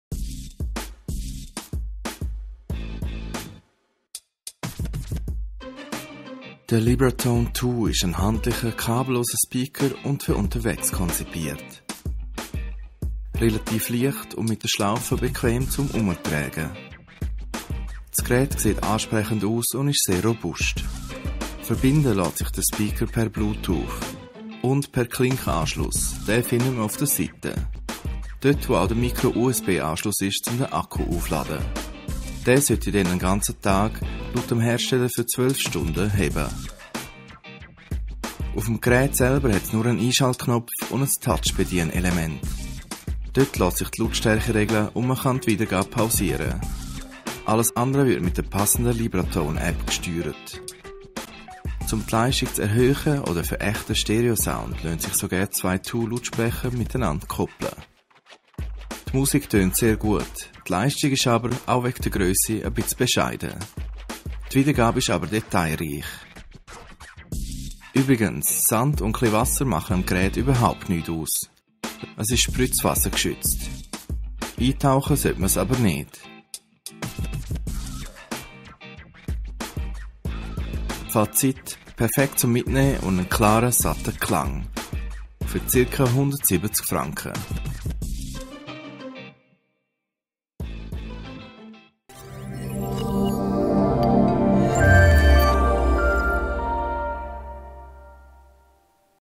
Mobile Soundanlage Libratone Too Im Test
Der Bluetooth-Lautsprecher Libratone Too liefert für seine geringe Grösse einen angenehm klaren, satten Klang und eignet sich perfekt zum Mitnehmen, um unterwegs eine mobile Soundanlage einzurichten.